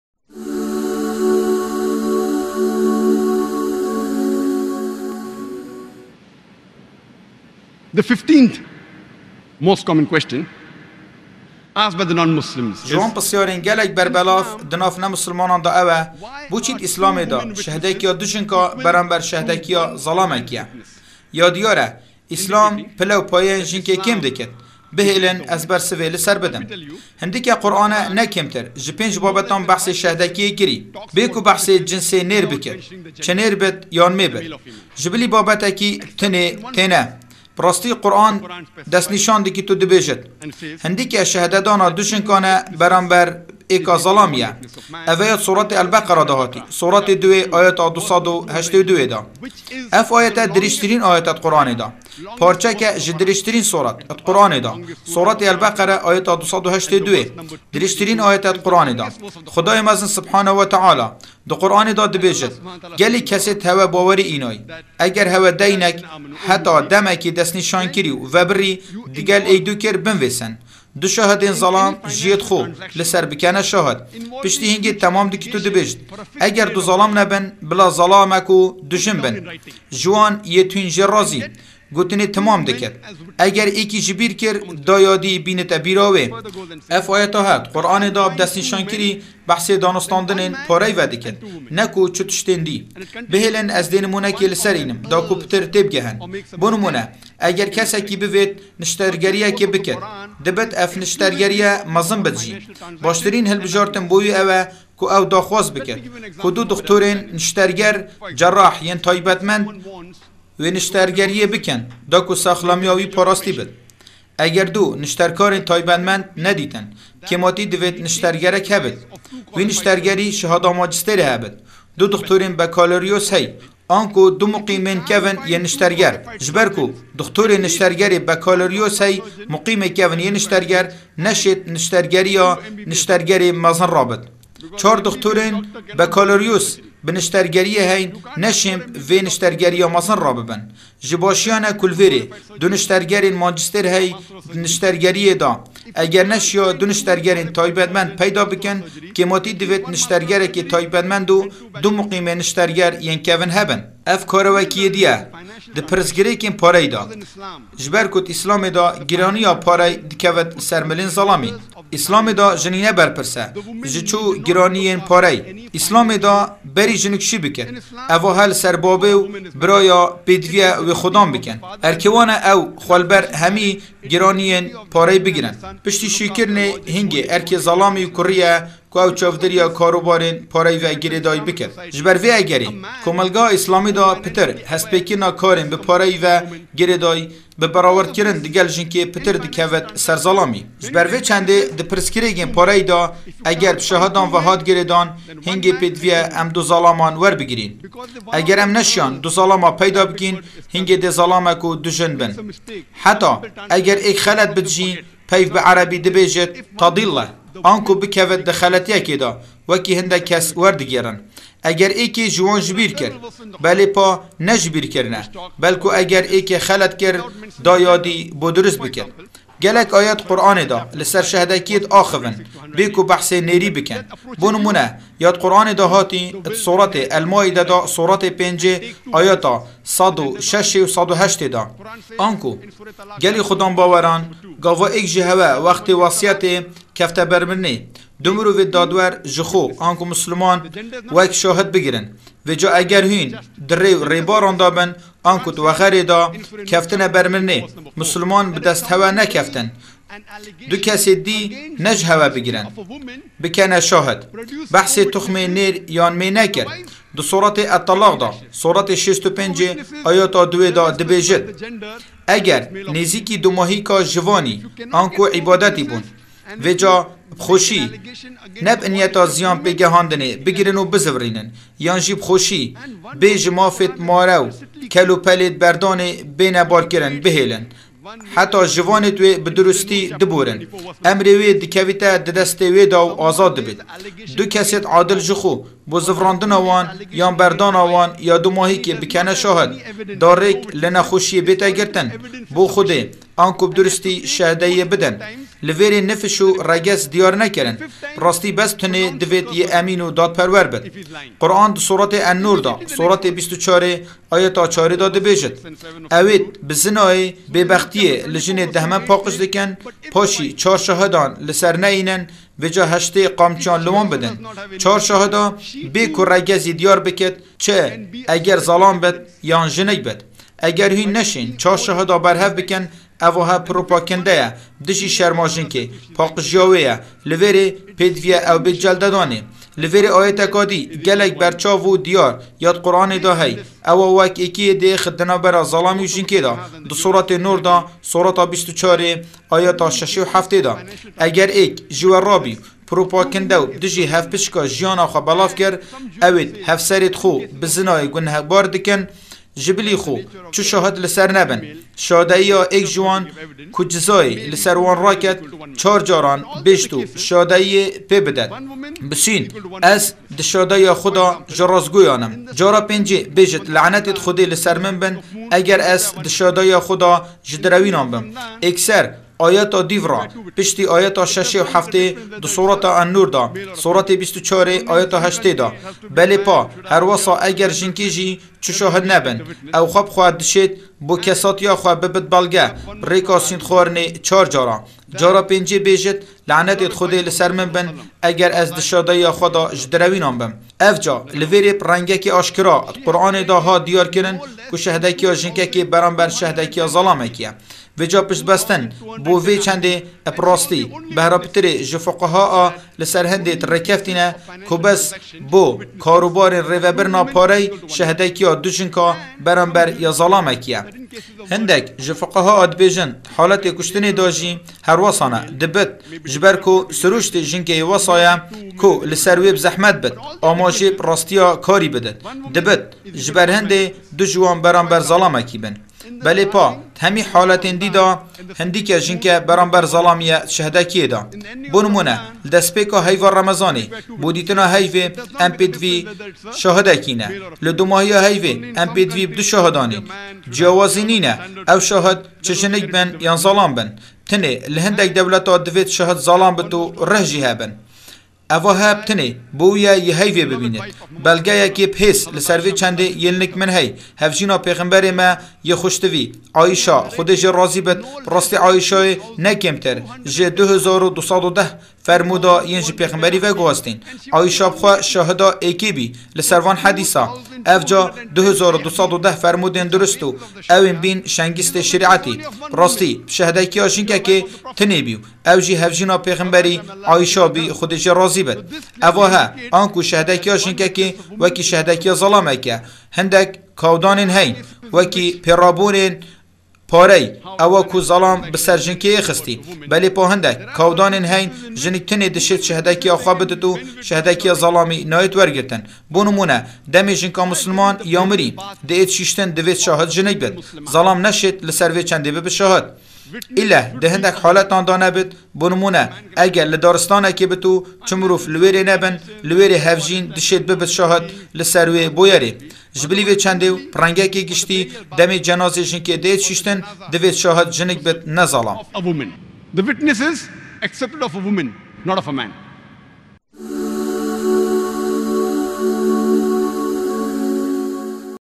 بەرسڤێن دکتور زاکر نایکى - 7
بوچى شەهدە کیا دوو ژنان دئیشلامێ دا بەرامبەر یا زەلامەکێ یە ؟
ـــــــــــــــ
Instegram⬇️